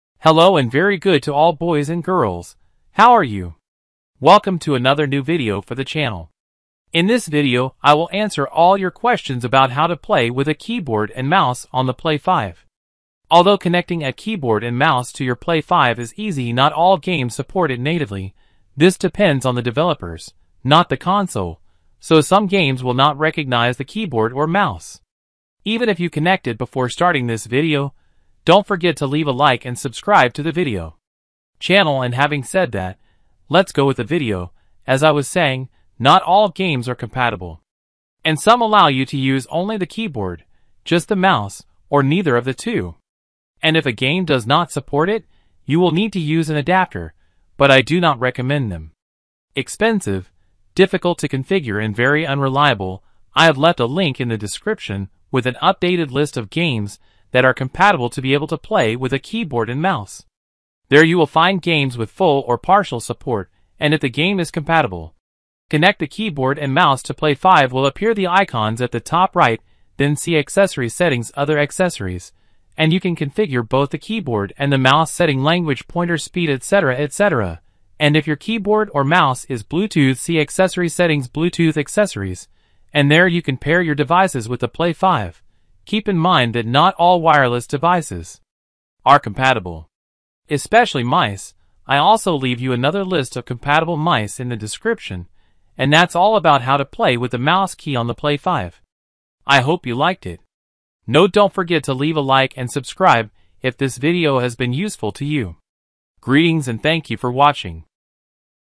How to Play Any Game with KEYBOARD and MOUSE on PS5 Is it possible?
In this video I show you how you can play any game with a keyboard and mouse on PlayStation 5. Can you really use the keyboard and mouse with all games? I teach you how to discover if a PS5 game is compatible with a keyboard and mouse, how to connect them via USB or Bluetooth wireless connection, and how to configure the keyboard and mouse on the PS5.

List of all games compatible with keyboard and mouse on PS5

You can set the keyboard language to your liking to have the best key layout, its delay in repeating keystrokes or even the speed of the mouse pointer.